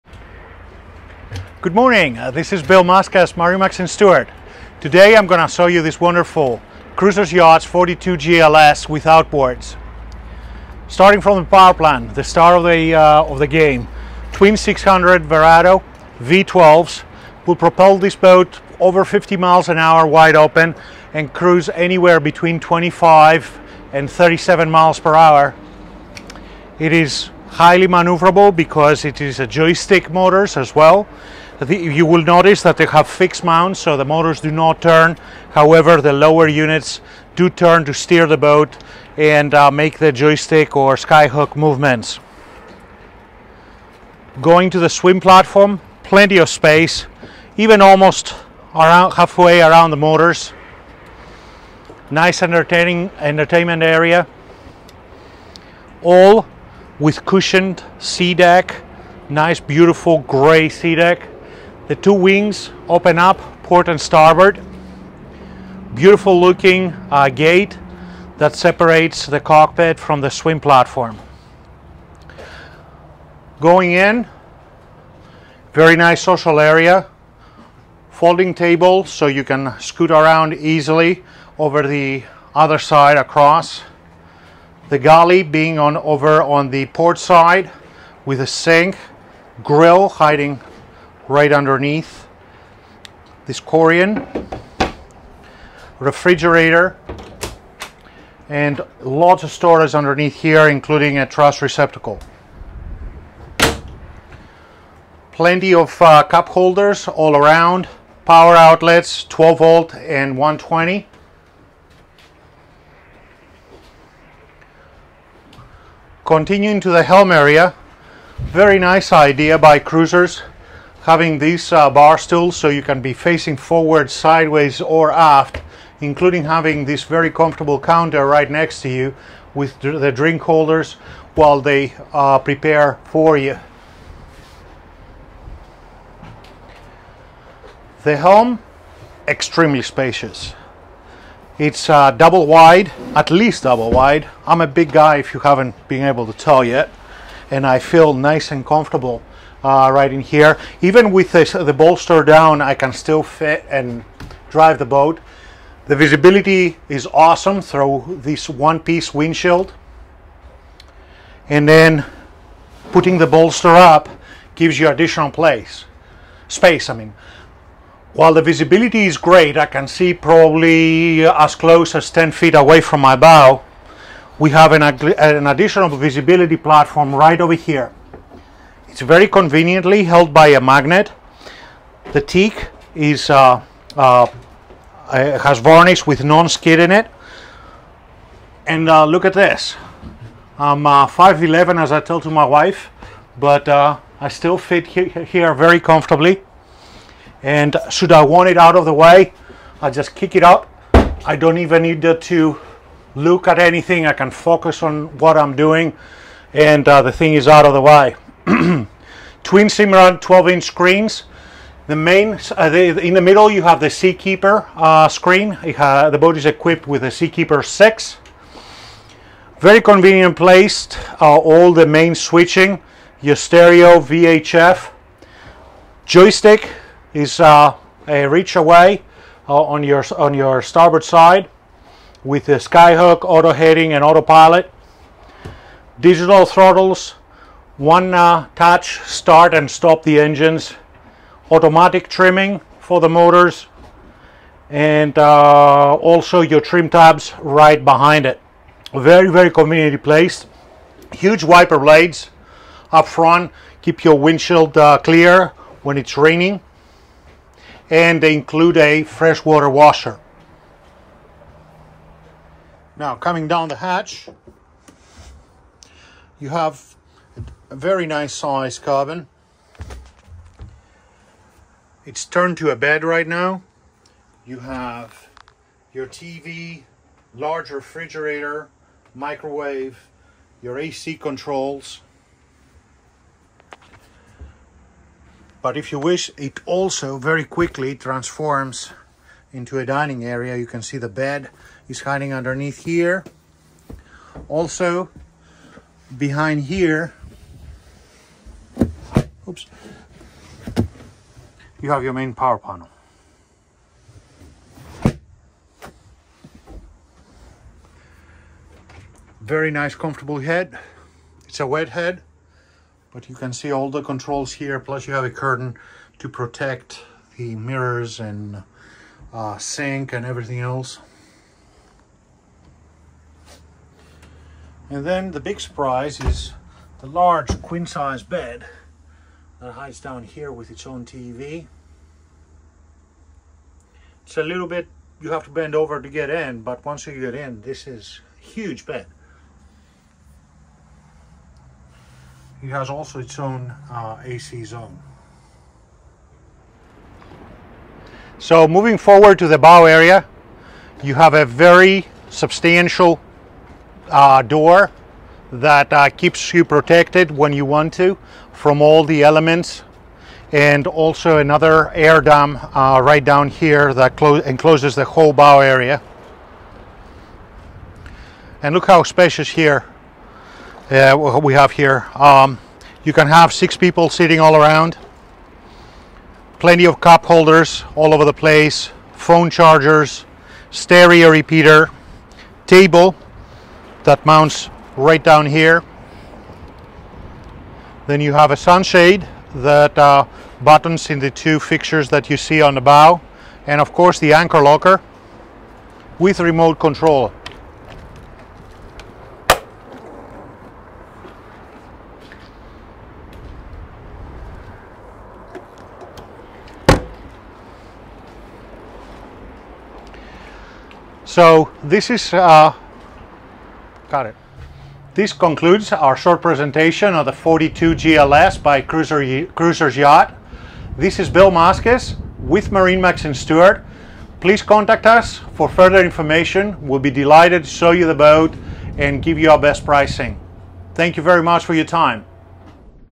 AMERICAN MADE | 2023 Cruisers Yachts 42 GLS Outboard | MarineMax Stuart
If adventure is your main goal when it comes to boating, look no further than the fast, stylish, and fun 42 GLS outboard for sale near you. She has the space, speed, and amenities to turn heads the world over. Options include:

Twin Mercury 600HP with Joystick piloting, white
White hull
Gray cockpit seating
Gray interior color, Ebony oak interior wood
Black standard canvas
Black hardtop
Black shade retractable, cockpit
Swivel, gray cockpit seat
SeaKeeper 6
Batteries, absorved glass mat/AGM, OB 600
9KW Diesel generator w/sound shield
Central vacuum system, 120V
Macerator w/overboard discharge
Cockpit wash down system, freshwater
Helm seat storage
Storage, w/refrigerator, wet bar
Underwater lights
VHF radio, Simrad